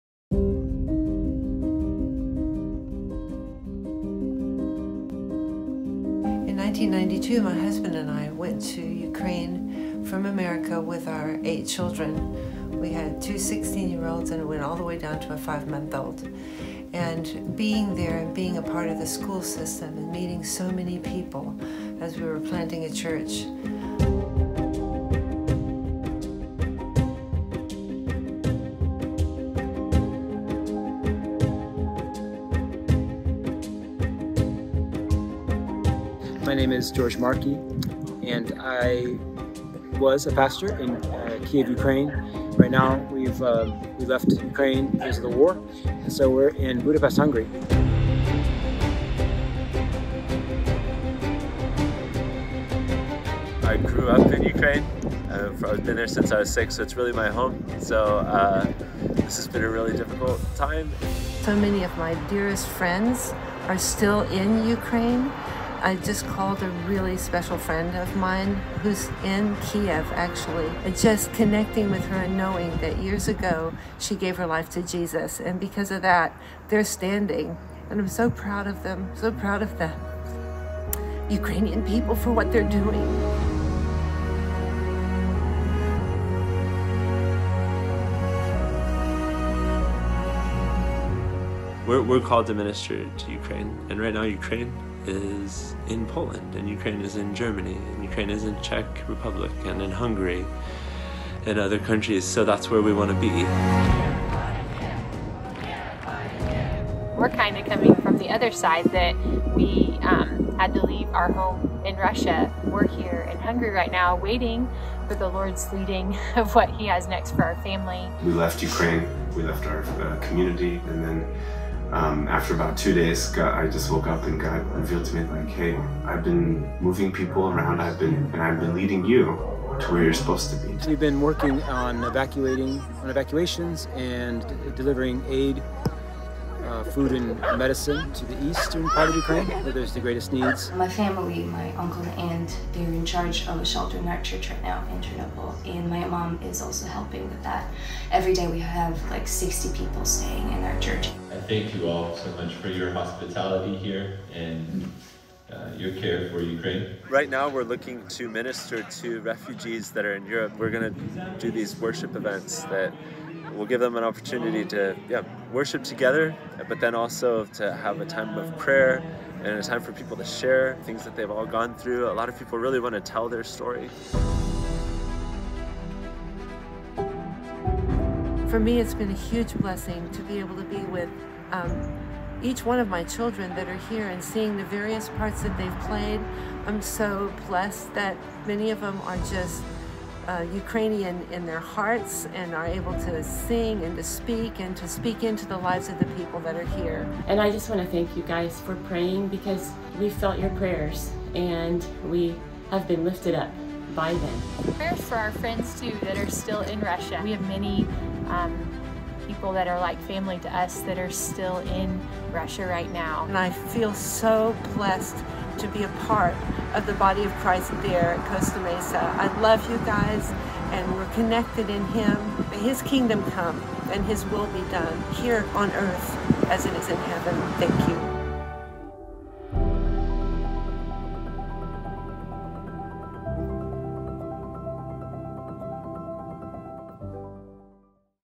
Refugees, Family, Missionaries, & Church Planters
As the war rages on in the Ukraine, for one family of missionaries these events are deeply personal. The Markey family have been a church planting family in the Ukraine and its surrounding areas since 1992. Hear updates from the family on the fallout from the war in Ukraine. They have fled from Kyiv, Russia, and beyond. Now as refugees themselves, they are seeking to minister to the Ukrainian people wherever they are.